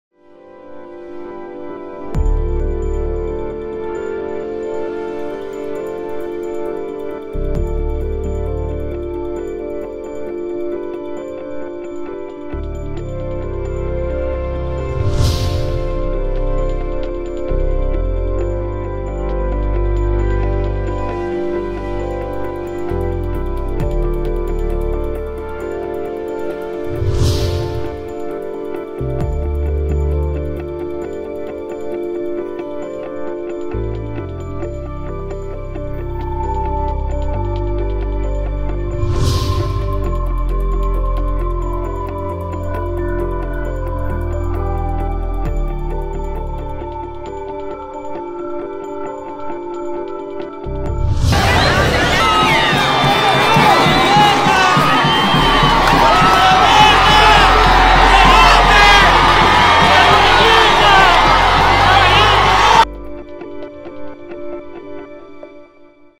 Las5DelDía | 8 octubre 2019
Manifestantes indígenas se toman el Parlamento de Ecuador y piden la salida del presidente Lenin Moreno, Poder Judicial reelige a la magistrada orteguista Alba Luz Ramos como presidenta. Ampliá estas y otras noticias aquí: ow.ly/Mhfe30pGNbT